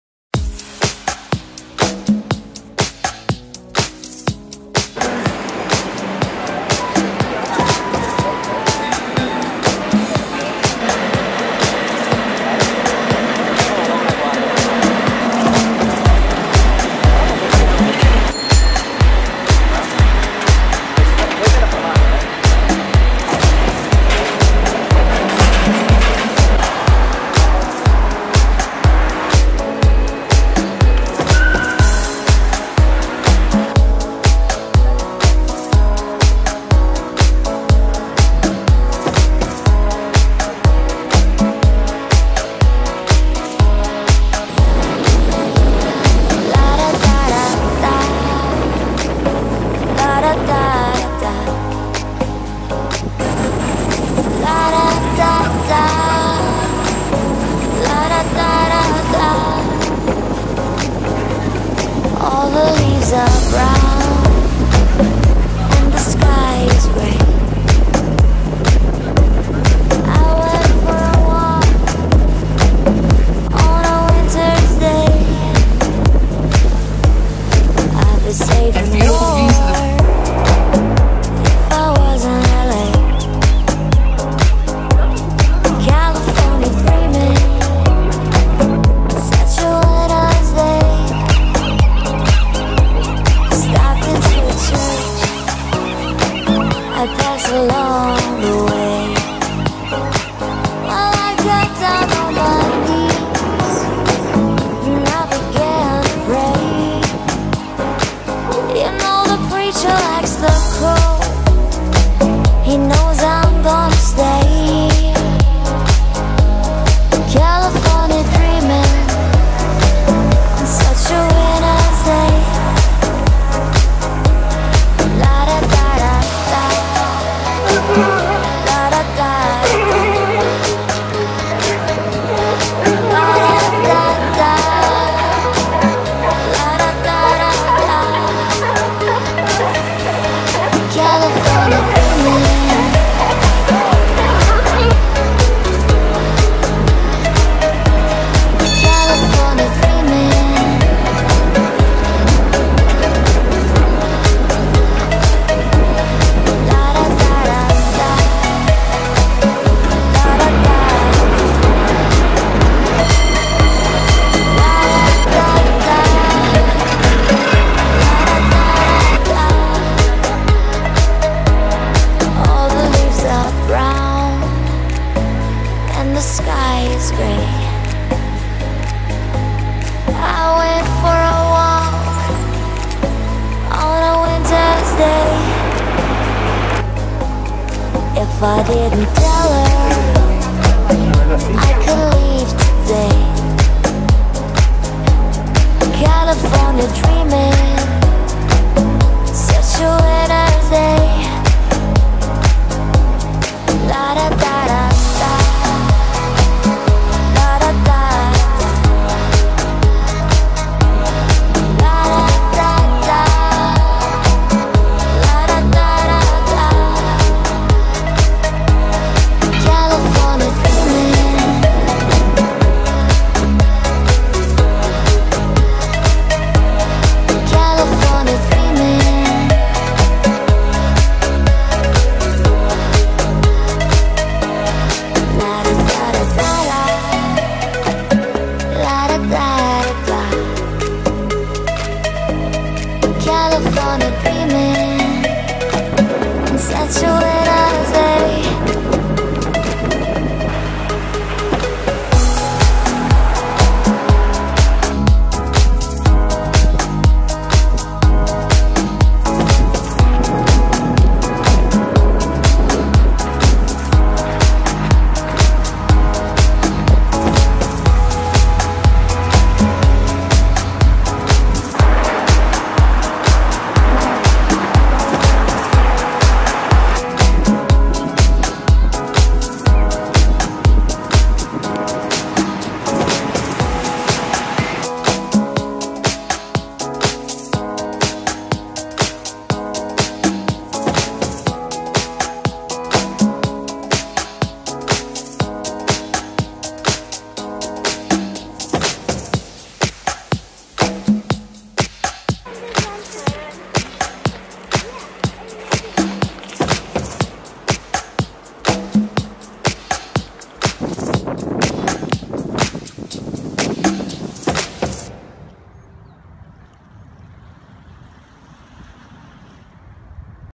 San Francisco California Roadtrip
Startort des Roadtrips durch Kalifornien und Nevada. Danke an das Reisebüro USA Reisen aus Berlin!